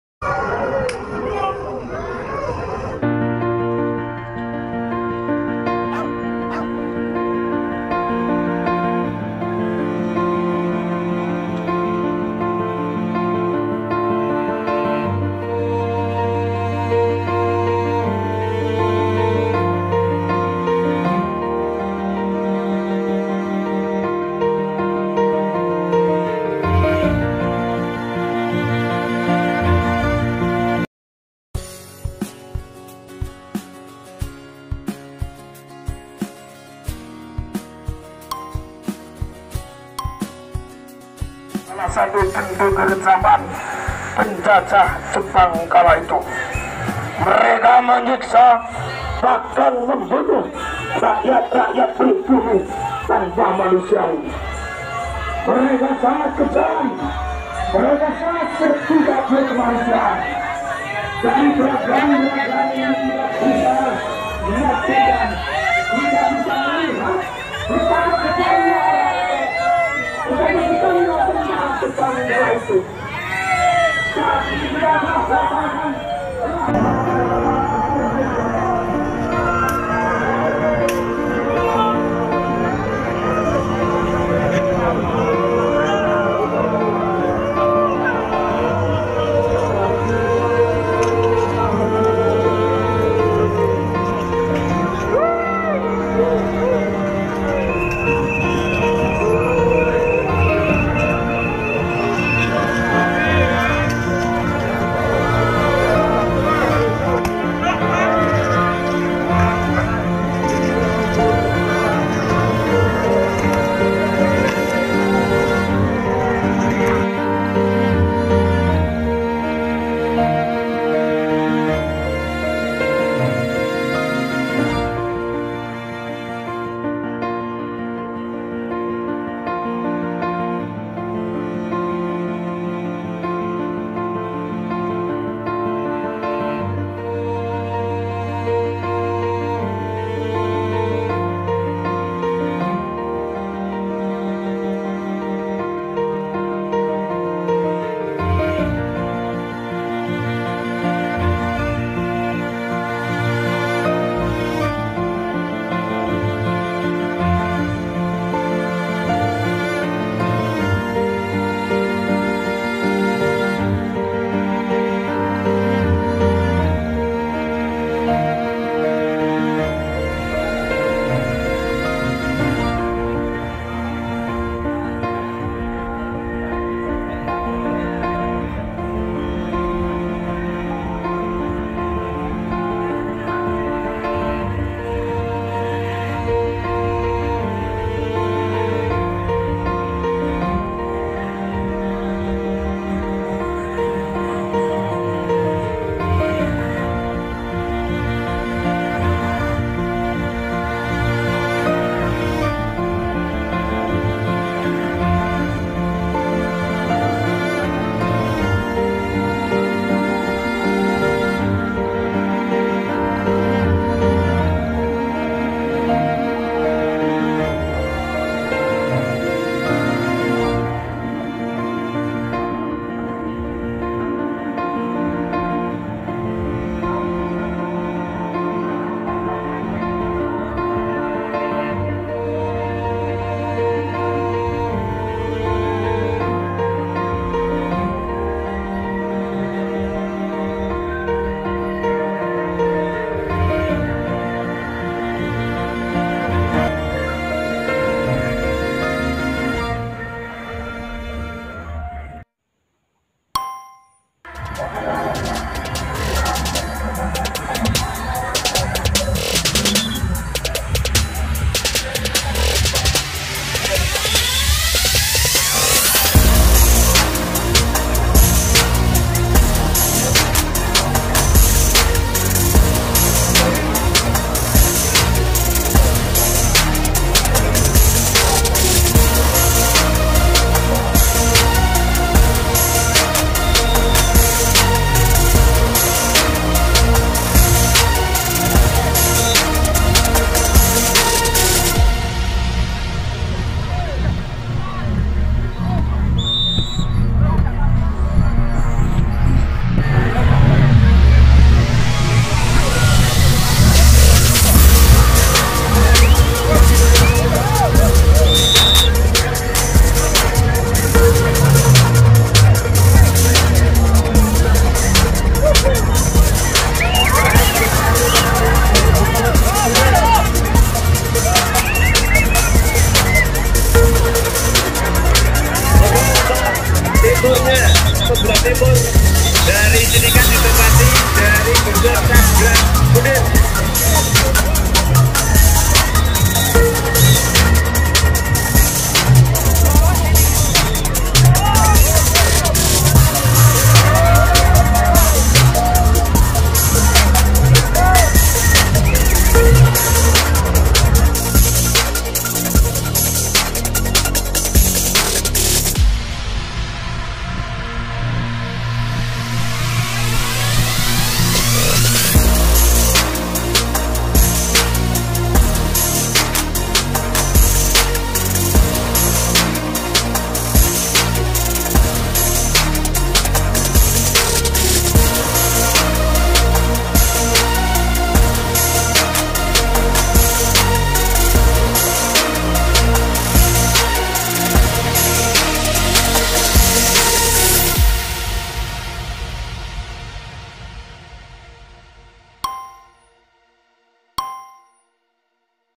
🔴(LIVE) Drama Kekejaman Penjajah Terhadap Indonesia 😱||Karnaval Desa Ropoh
karnaval 17 Agustus menceritakan kisah pada masa penjajahan dahulu sebelum Indonesia merdeka dan kreatif || barisan karnaval HUT RI ke-77 di desa Ropoh, kec kepil,kab Wonosobo